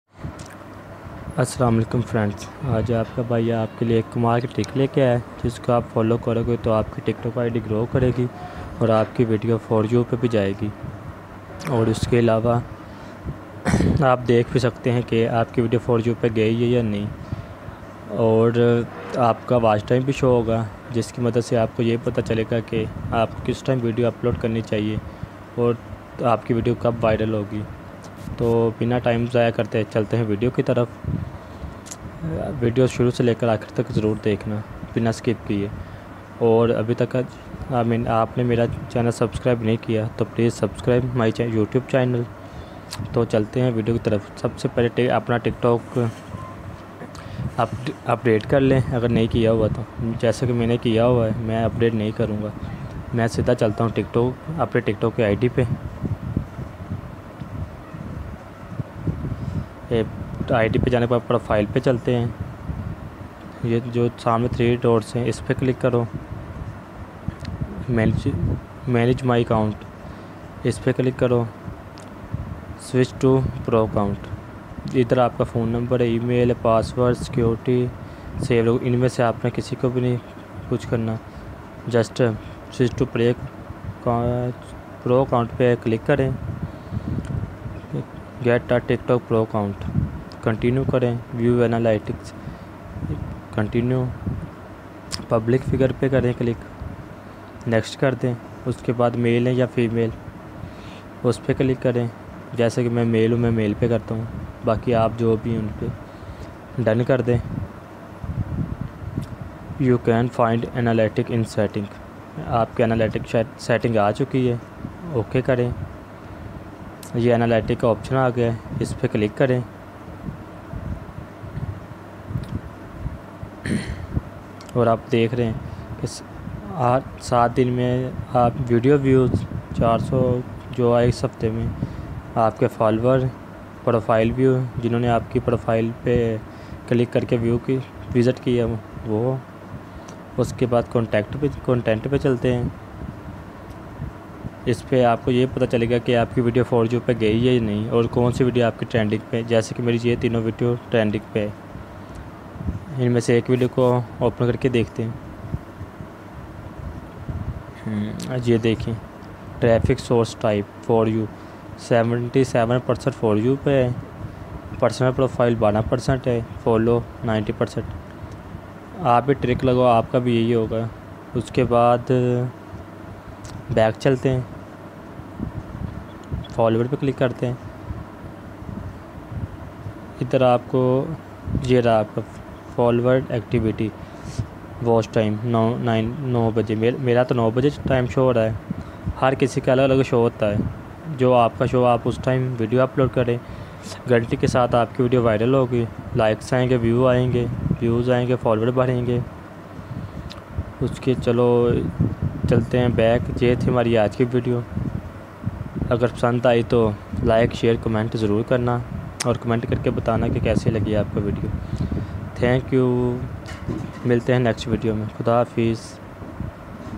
How To Switch Tiktok Pro Account On Tiktok 2020
How To Switch Tiktok Pro Account On Tiktok & Benifits Of Tiktok Pro Account.

Ap b apna tiktok account pro kro or dekho k us se apko kya kya faide hote, to jldi se kre account pro is se apko aik faida ye b hoga k ap analytic ka option hoga, jis se apna watch b dekh skte h or tiktok k us btaye hue time pe video upload karo or dekho kese apki video viral ho h is se ap Views, Likes & Followers b bhare ge, or to or ap ye b dekh skte h k apki kon konse video foryou pe gai h k ni or konsi ni gai, ap ye b dekh sakte h k apki konsi video trending pe or konsi, to islye jldi ap tiktok account pro.